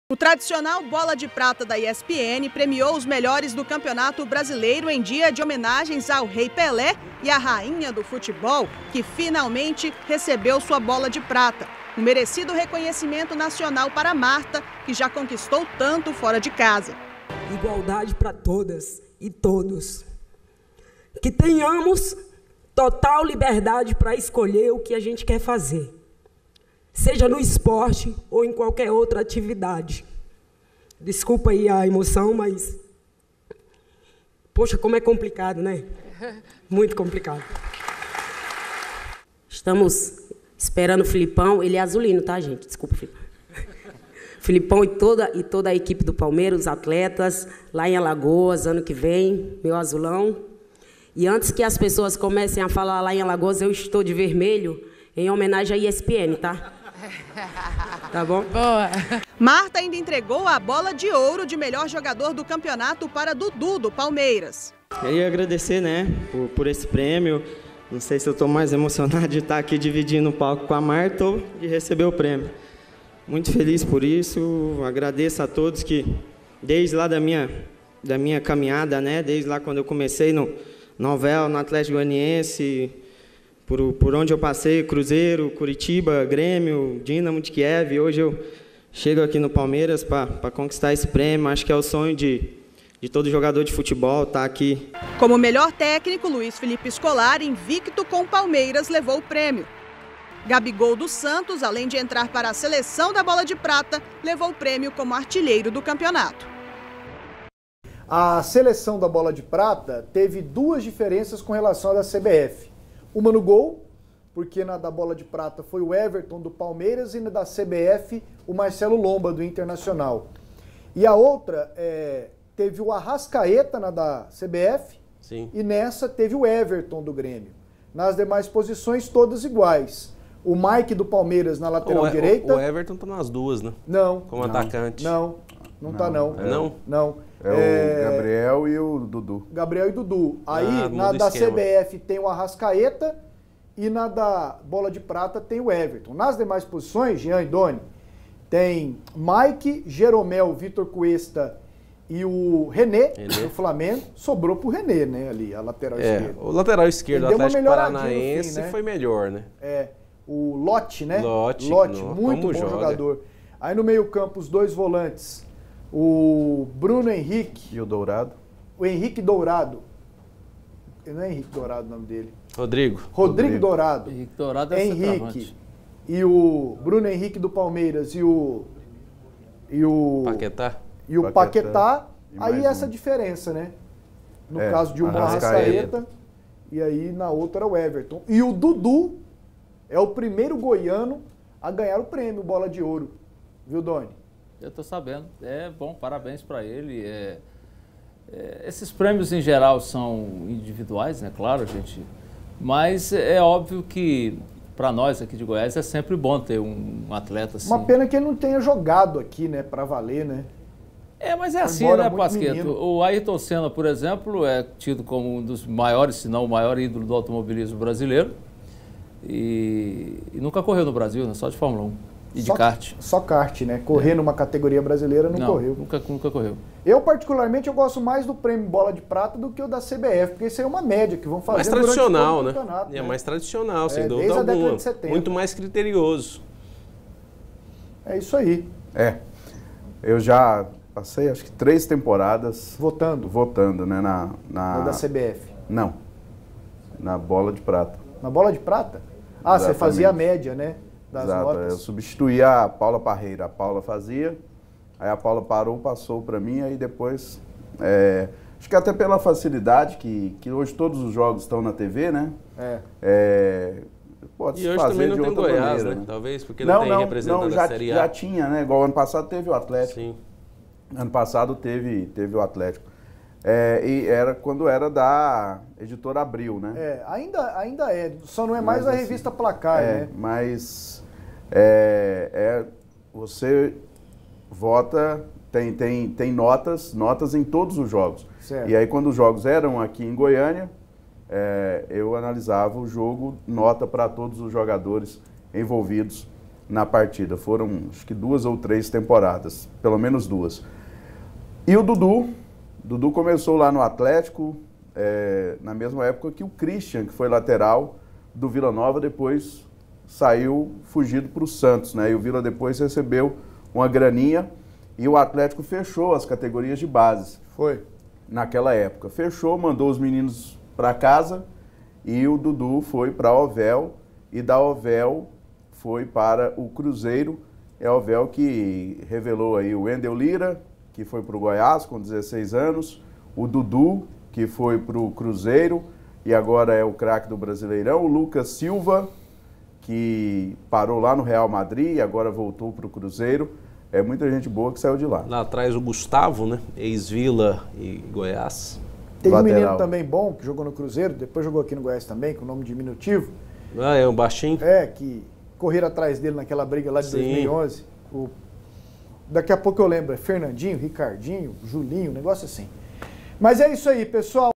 Premiação ESPN
Dudu, do palmeiras, é eleito o melhor do brasileirão;